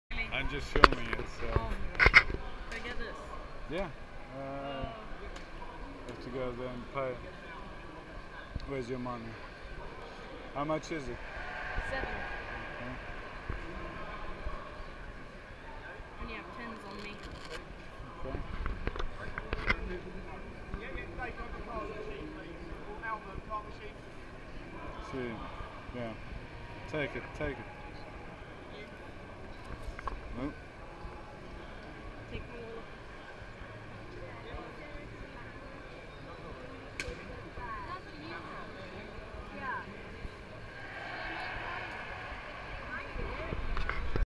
Josh at Star Trek Destination 3
Josh buying a few things at the convention.